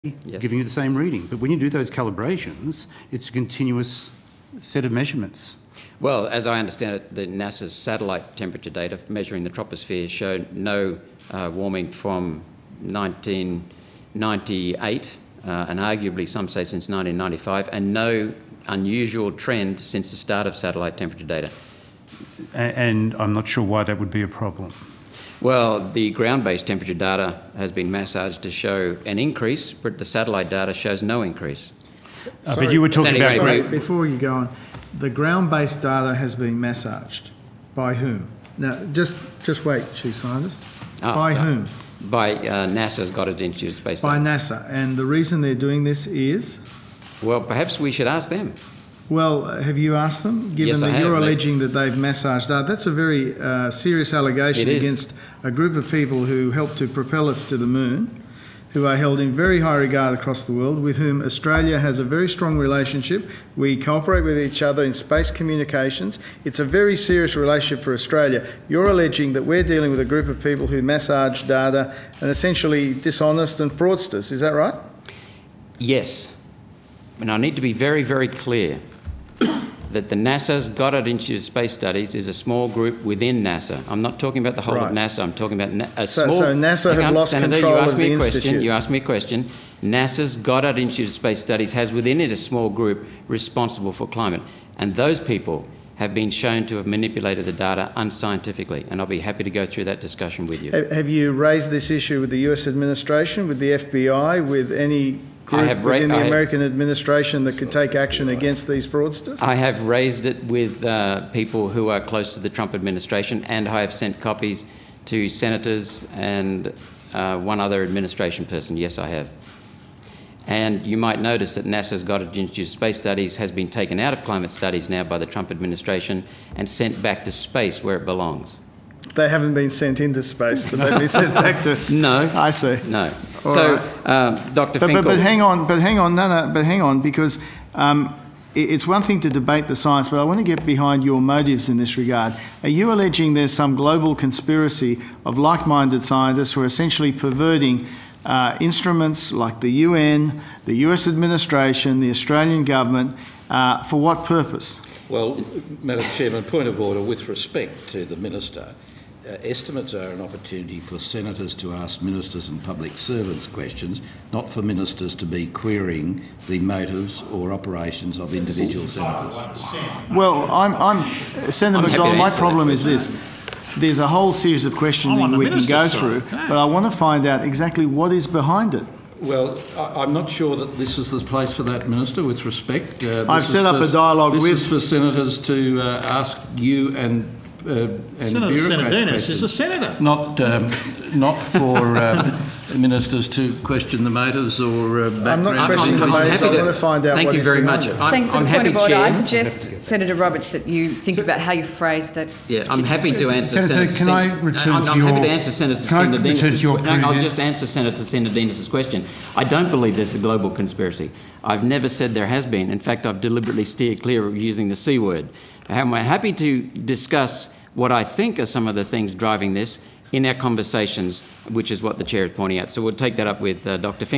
Australia's Chief Scientist and Science Minister Face Off Against Climate Change Doubter
Australia’s Chief Scientist and Science Minister faced off against a far-right Senator who suggested in a Senate hearing that NASA’s Goddard Institute had manipulated climate science data to further a presumed agenda.

It was the second barrage of questions from One Nation’s Malcolm Roberts, who earlier attempted to challenge climate change data using a hoax penis article. While inquiring about how global temperatures were measured, Roberts told Chief Scientist Alan Finkel that he believed NASA’s Goddard Institute had “massaged” the numbers to give the impression of rising ground temperatures.

The Minister for Industry, Innovation and Science Arthus Sinodinos interjected and refused to allow Finkel to answer any more questions until Roberts clarified his line of questioning.

Source: Australian Parliament House